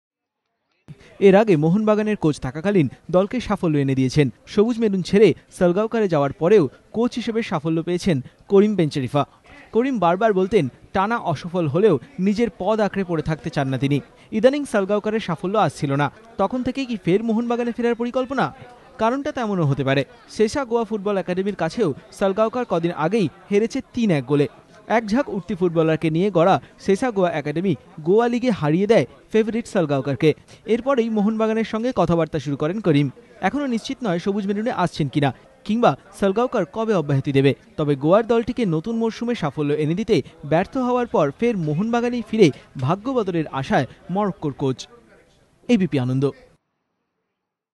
karim bencherifa's probable return to mohan bagan
karim bencherifa may return to mohan bagan as coach after failing to get success in goa football